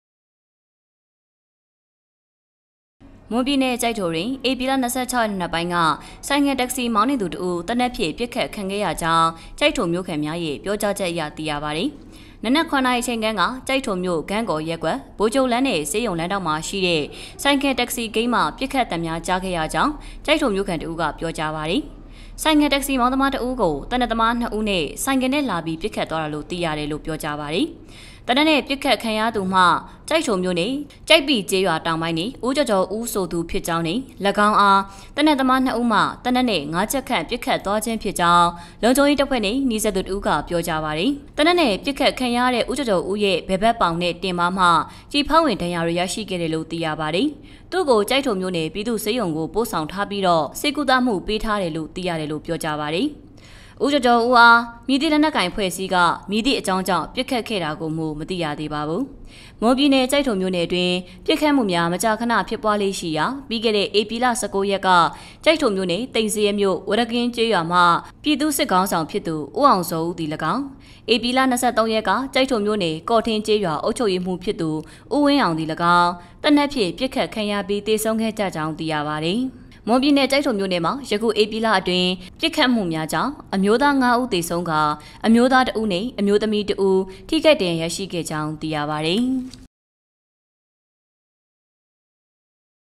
ကျိုက်ထိုမြို့တွင် ဆိုင်ကယ် Taxi မောင်းနှင်သူတစ်ဦး သေနတ်ဖြင့် ပစ်ခတ်ခံရ
မွန်ပြည်နယ် ကျိုက်ထိုတွင် ဧပြီ ၂၆ ရက်နံနက်ပိုင်းက ဆိုင်ကယ် Taxi မောင်းနှင်သူတစ်ဦး သေနတ်ဖြင့် ပစ်ခတ်ခံခဲ့ရကြောင်း ကျိုက်ထိုမြို့ခံများ၏ ပြောကြားချက်အရ သိရသည်။